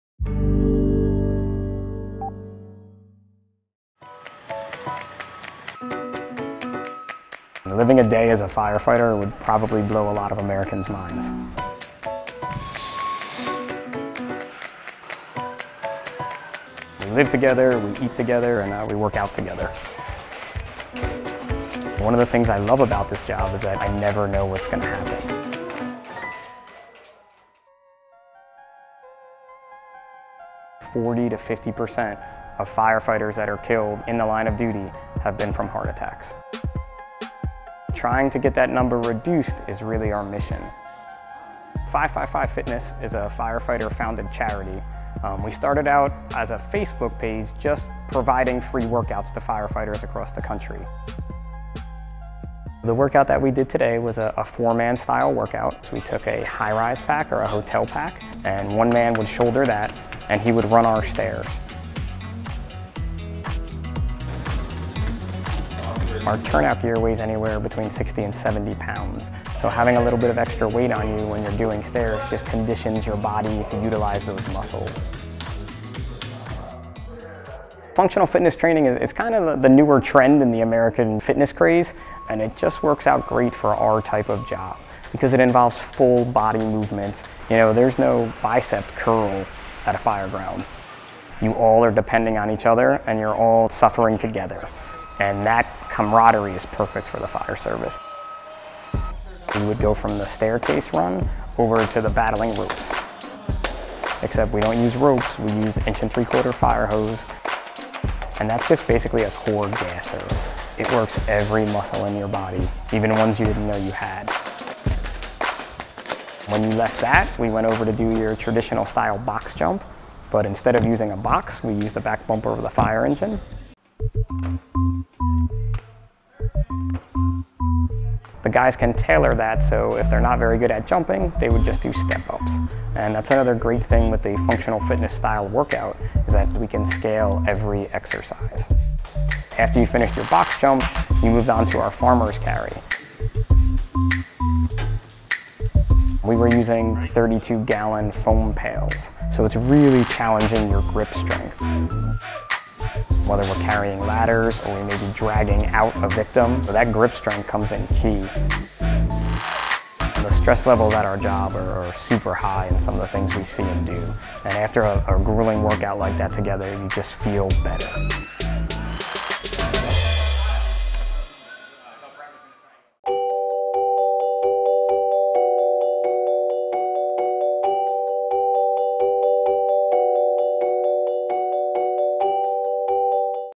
The Firefighter’s Workout | The New York Times
A workout designed to help firefighters stay heart-healthy focuses on functional fitness exercises like stair climbing. Here’s how members of the New Brunswick, N.J., fire department stay fit.

Produced by: Colin Archdeacon